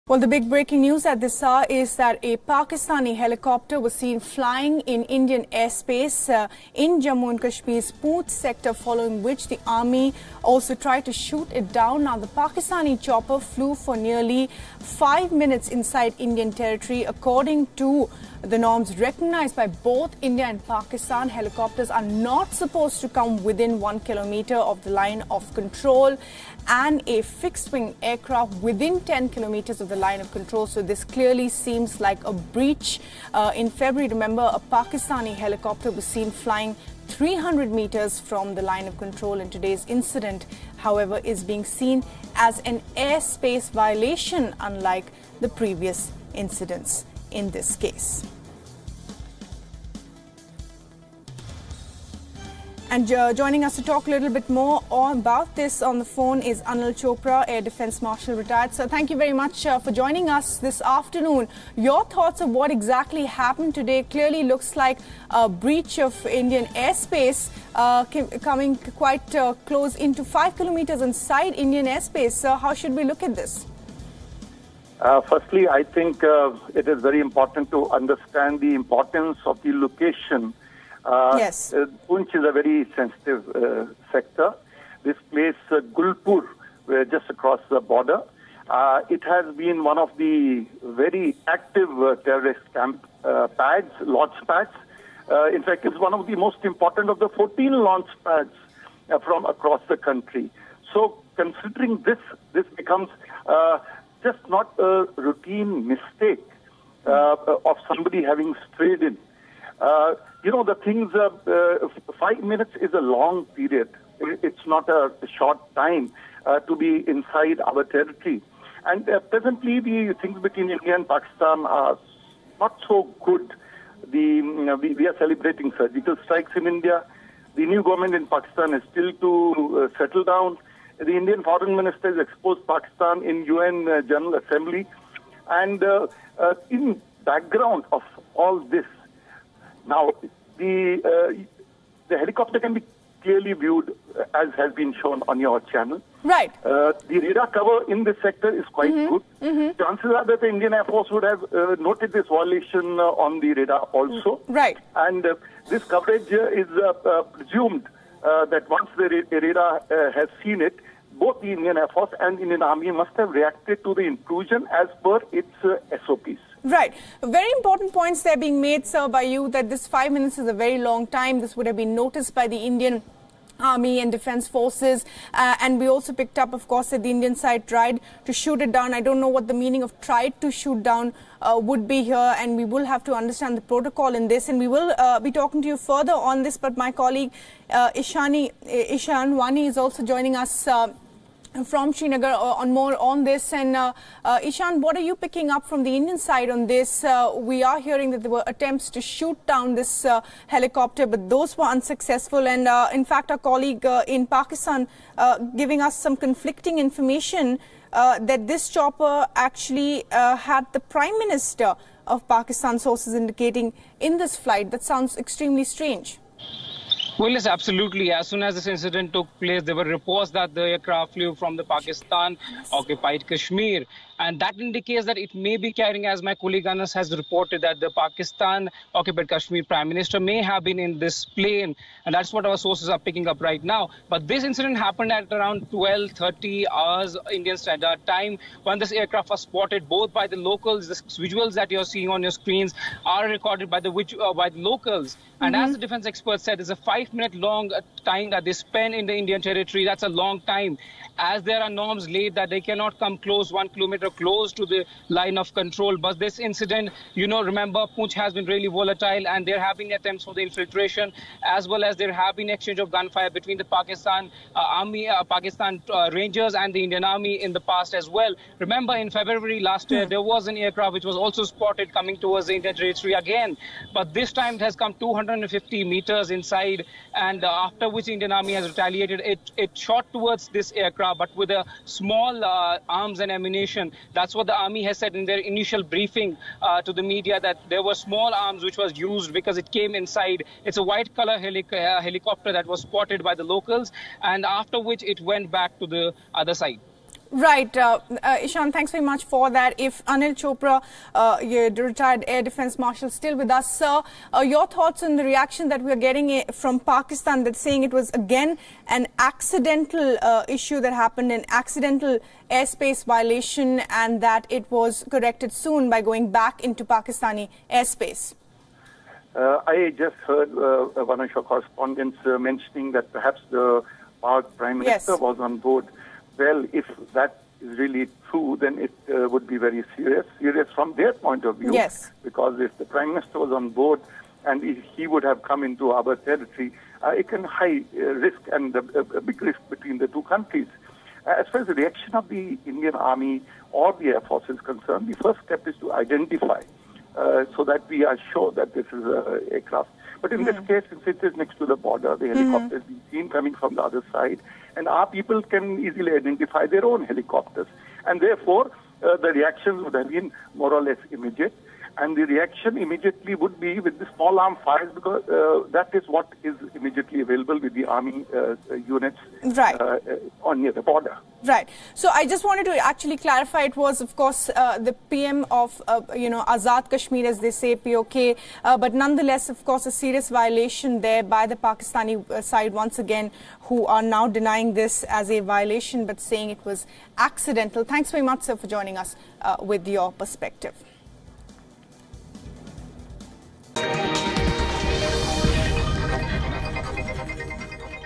Pakistani Chopper violates Indian Airspace
A Pakistan chopper was seen flying at Jammu and Kashmir's Poonch, following which the Army tried to shoot it down, people with direct knowledge of the matter said